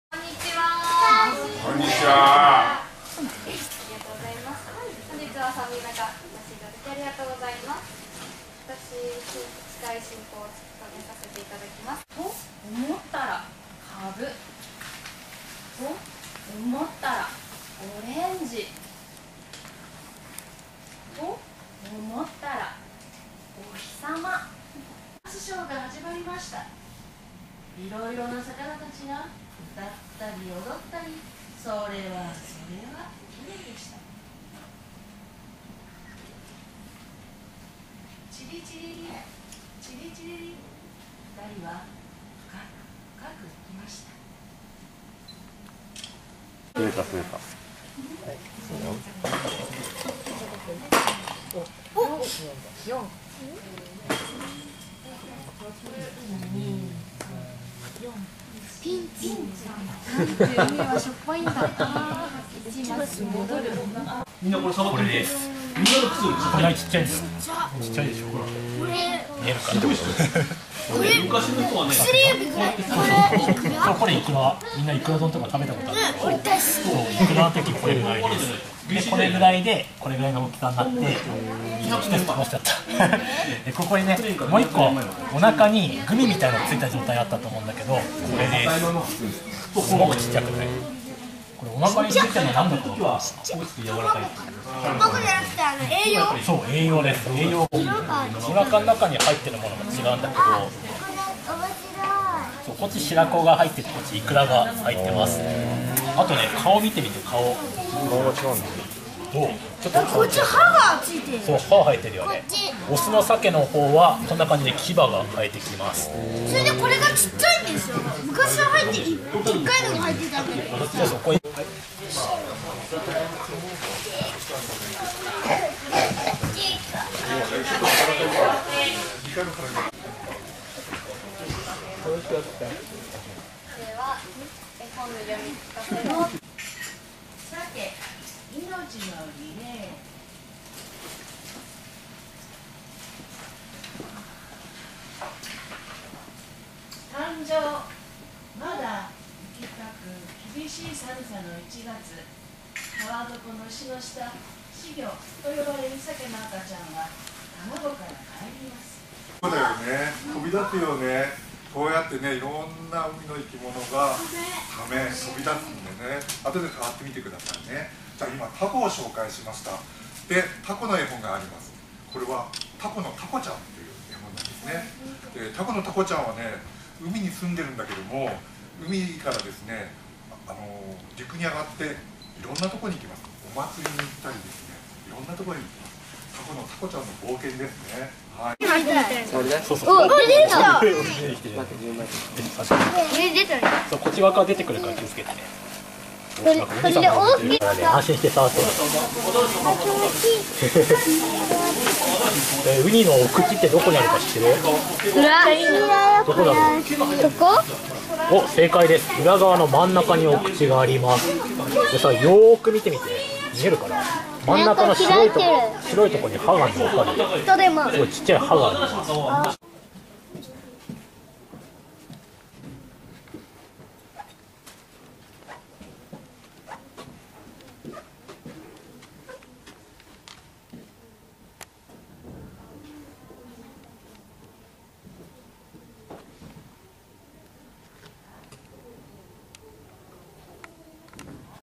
小樽図書館と水族館を繋ぐマジプロイベント！
小樽商科大学マジプロ「市立図書館の活性化」チームが企画して、12月11日(日)10:00から12:00まで、市立小樽図書館(花園5・鈴木浩一館長)2階視聴覚室で、「すいぞくかんがやってくる！」を開き、親子20名が参加。図書館と水族館が共に参加協力する初めての試みとなった。